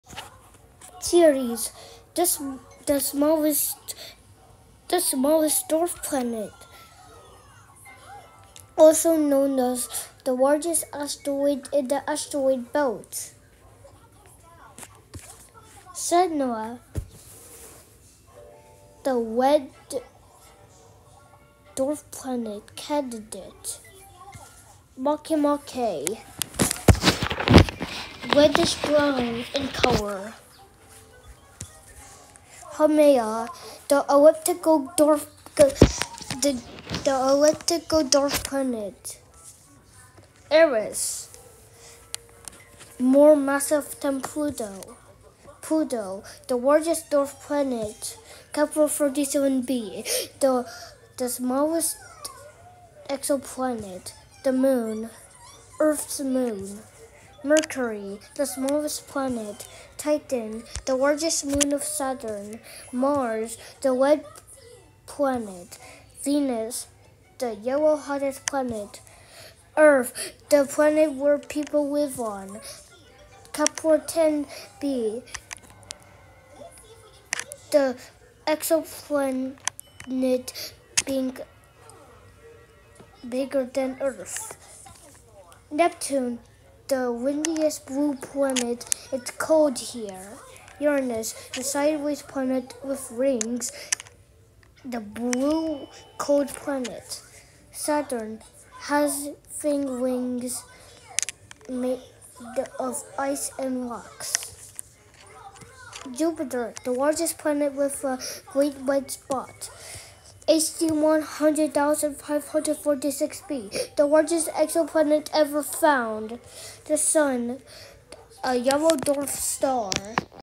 Universe Size Comparison: Moons, Planets, Stars, and More
Ceres: The smallest dwarf planet. It is the largest asteroid in the asteroid belt.
Sedna: A red dwarf planet candidate, but red like Mars, a neptunain object.
Makemake: The red brown dwarf planet.
Haumea: A oval shaped dwarf planet that have the fast rotation.
Eris: The dwarf planet more massive than Pluto.
Pluto: The largest dwarf planet.
Kepler 37b: The smallest exoplanet.
The Moon: Also known as Luna, earth’s moon.
Mercury: The smallest planet.
Titan: The largest known moon of Saturn.
Mars: The red planet.
Venus: The hottest planet.
Earth: The planet where people live on.
Kepler 10b: Among the medium exoplanets in the universe.
Neptune: The smallest gas giant, blue windiest planet, cold here.
Uranus: Sideways planet, with rings, the cold green blue planet.
Saturn: The planet with rings made of ice and rocks.
Jupiter: The largest known planet with the Great Red Spot.
HD 100546 b: Among the largest exoplanets ever found.
The Sun: A yellow dwarf star.